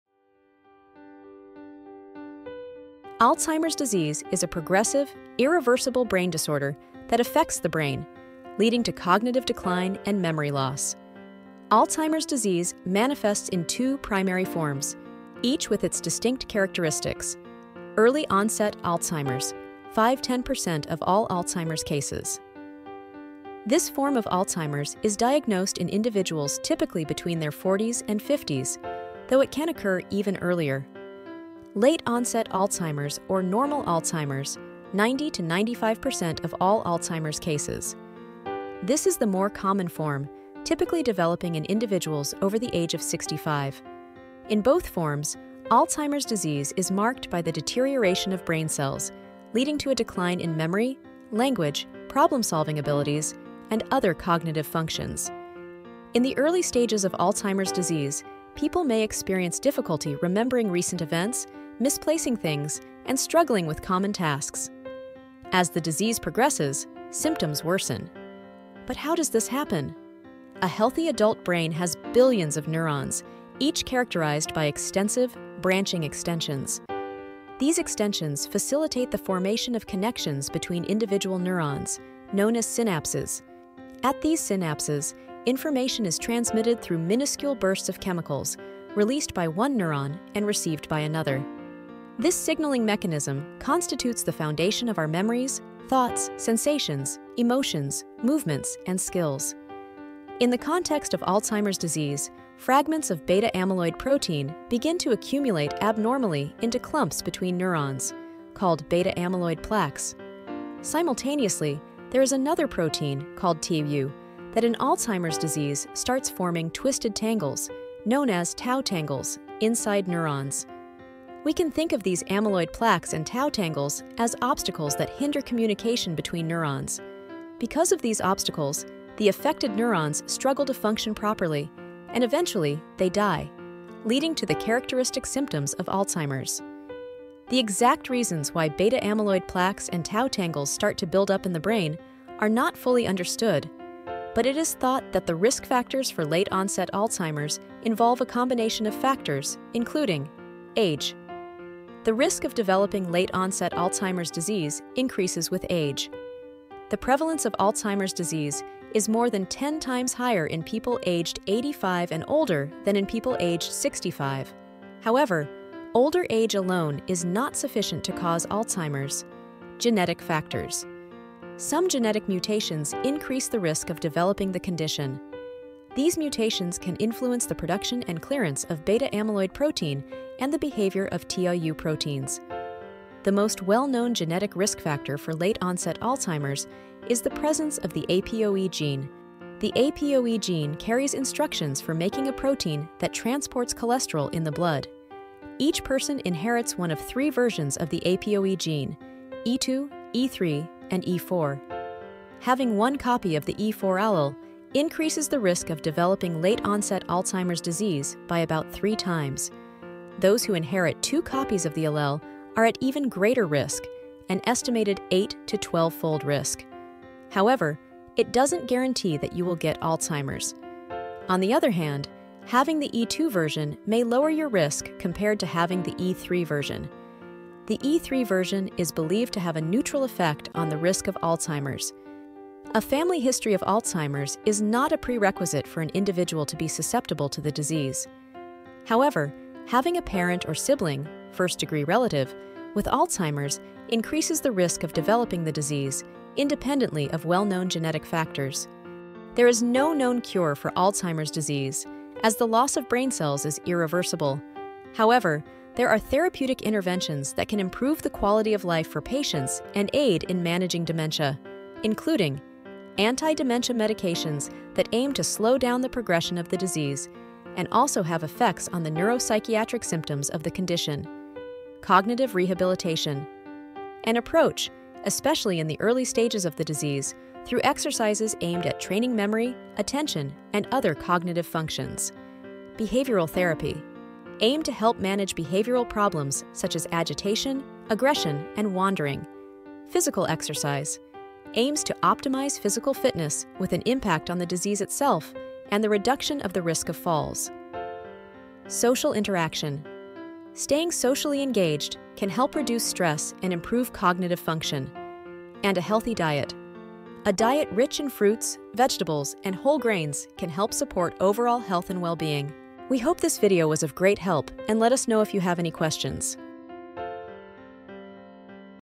Alzheimer’s Disease - meamedica DNA report
Short explanation of Alzheimer’s Disease and it genetic components